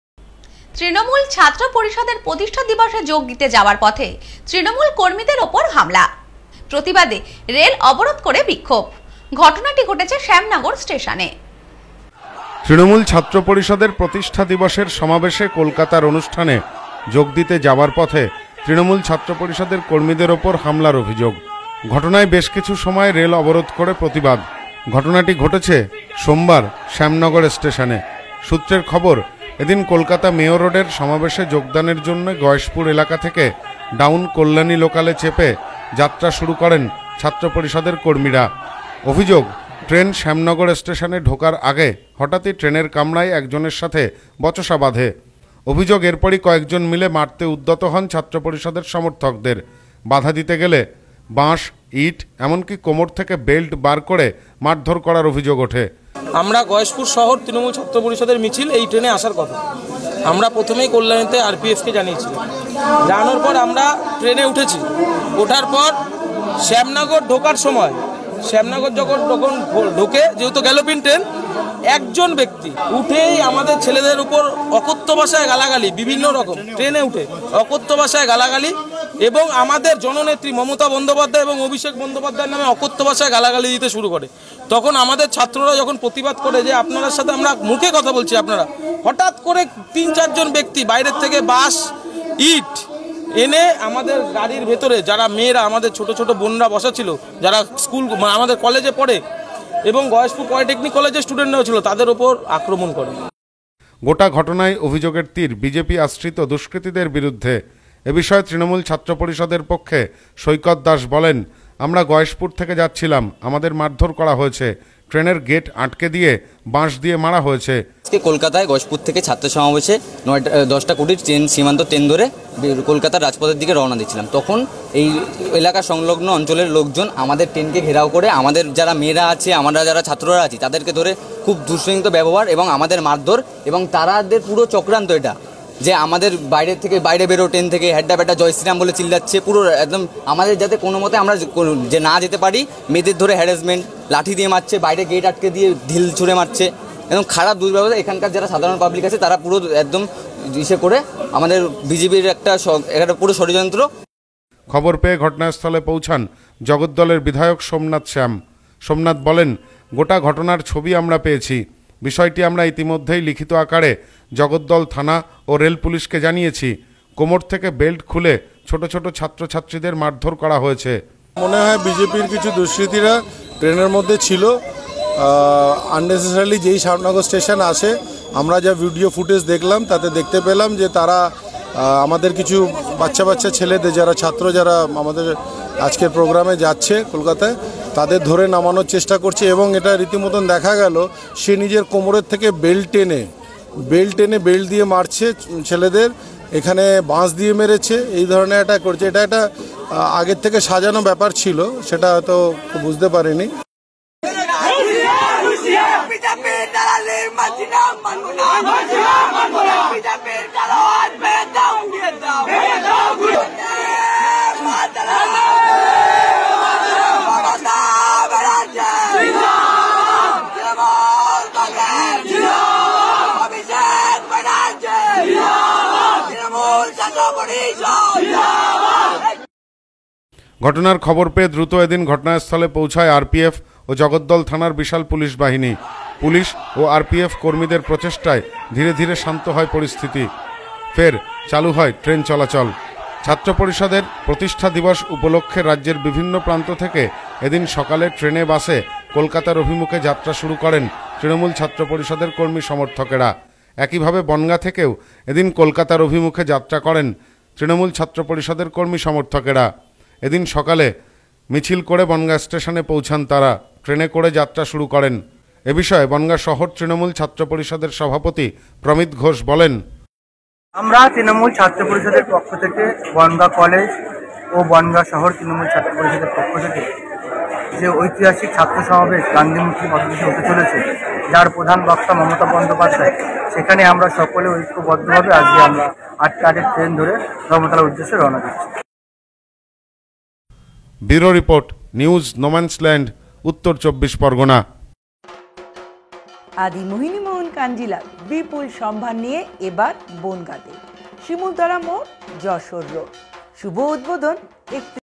তৃণমূল ছাত্র পরিষদের প্রতিষ্ঠা দিবসে যাওয়ার পথে তৃণমূল কর্মীর উপর হামলা ৷
তৃণমূল ছাত্র পরিষদের প্রতিষ্ঠা দিবসে যোগ দিতে যাওয়ার পথে তৃণমূল কর্মীর উপর হামলার অভিযোগ৷ প্রতিবাদের রেল অবরোধ করে বিক্ষোভ ৷ ঘটনাটি ঘটেছে শ্যামনগর স্টেশনে ৷